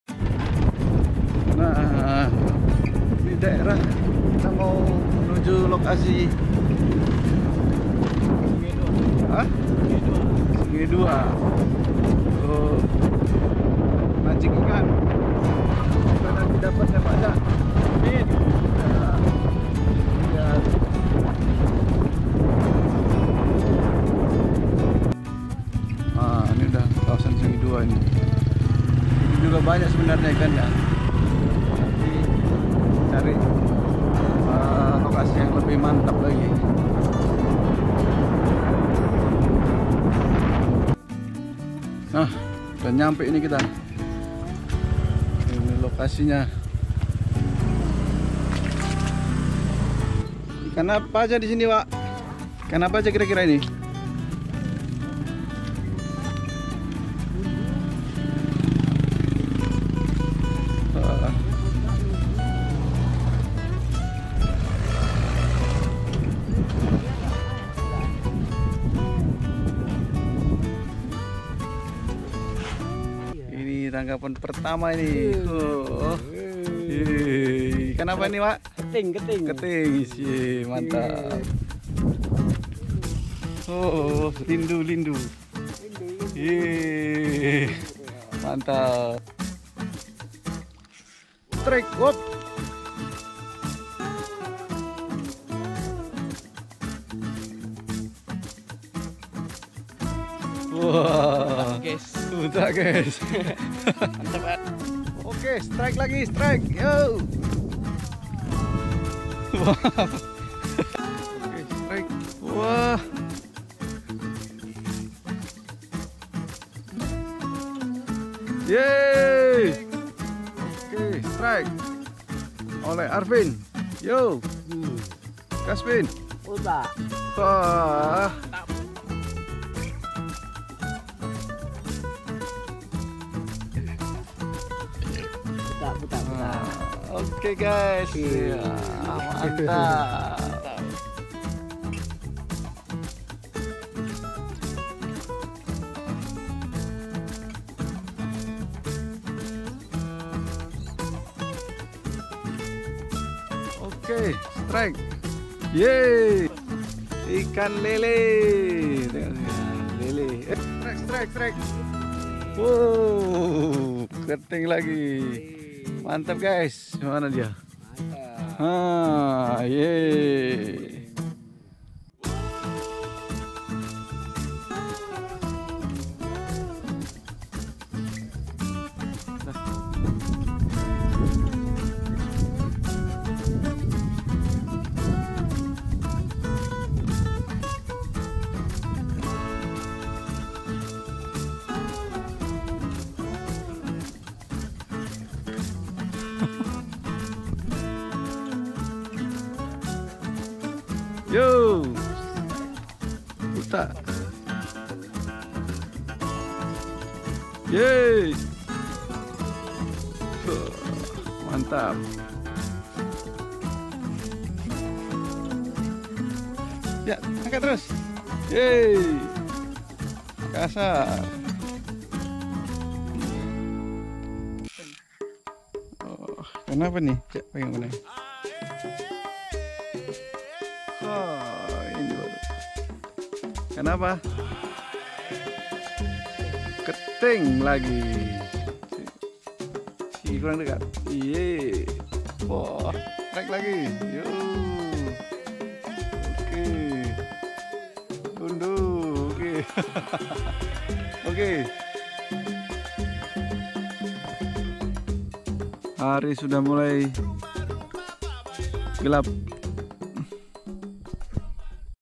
Strike Terus..!! Mancing di sungai dua tanjung balai asahan
strikenya terus terusan, lokasi mancing di parit bekas bekoan sungai dua tanjung balai asahan, ditengah perkebunan sawit.